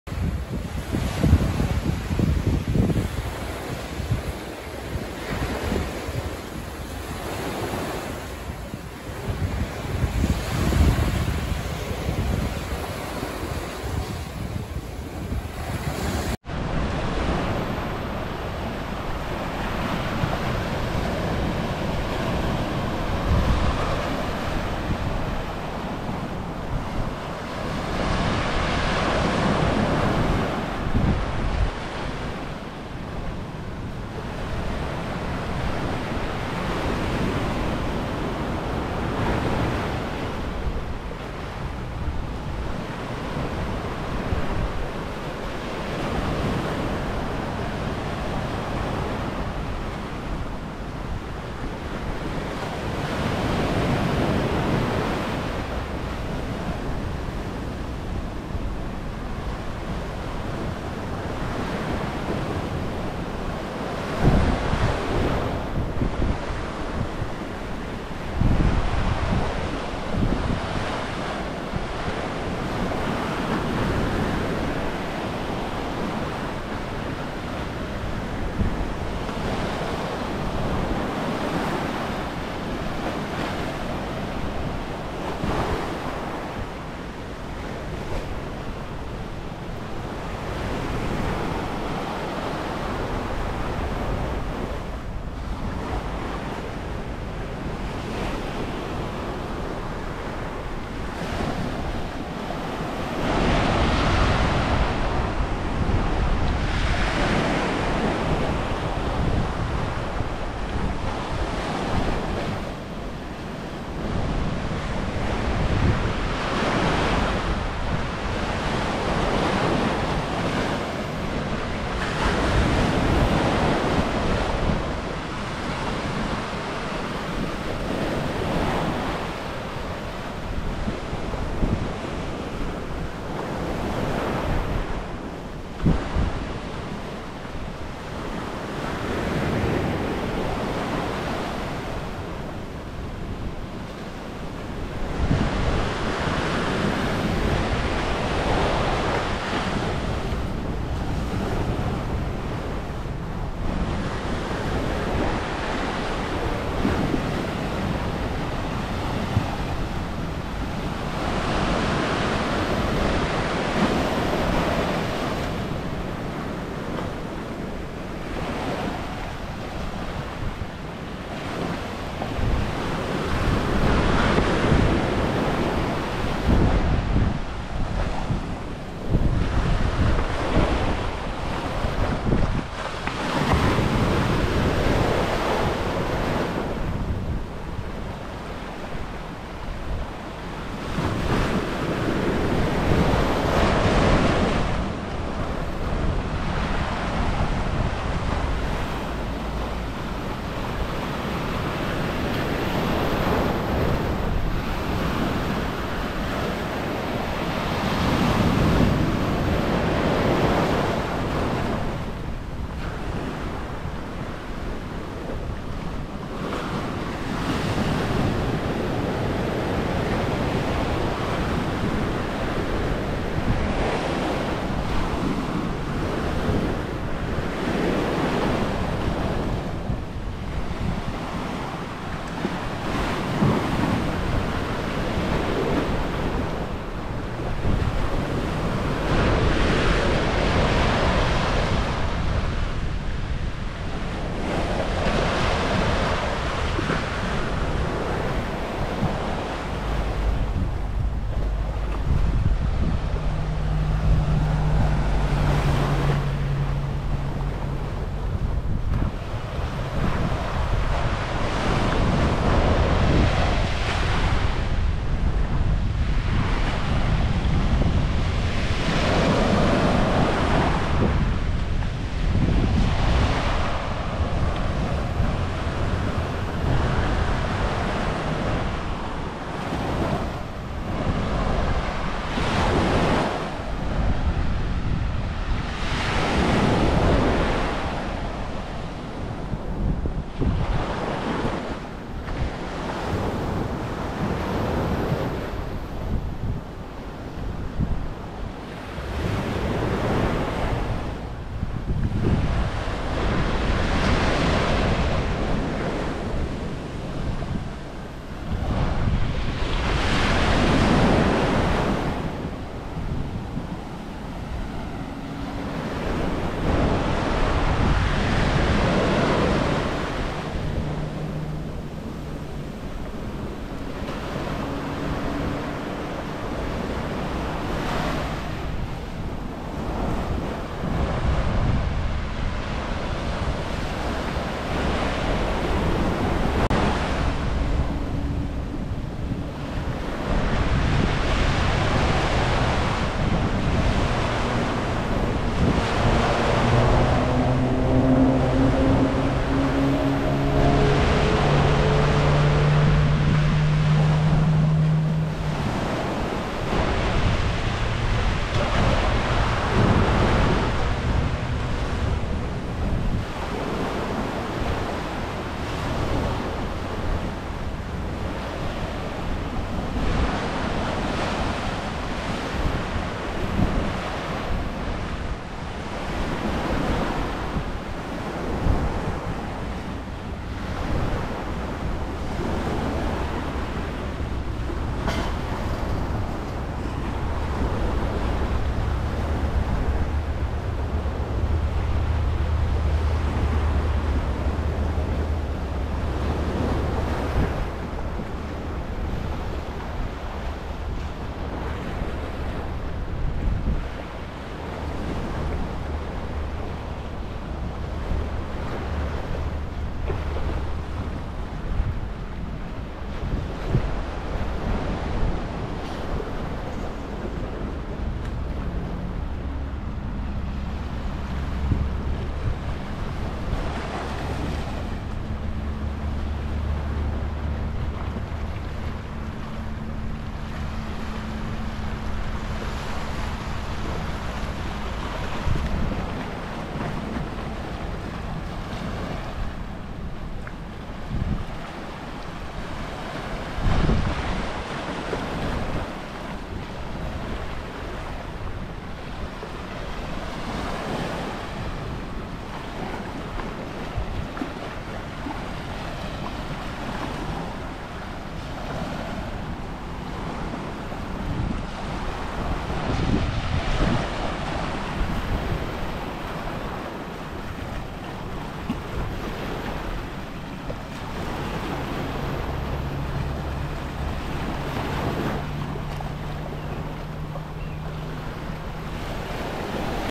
Go Walking! Makai Pier on Oahu Hawaii
Nice morning walk at Makai Pier by Makapuu Beach on Oahu Hawaii.

4K  ハワイ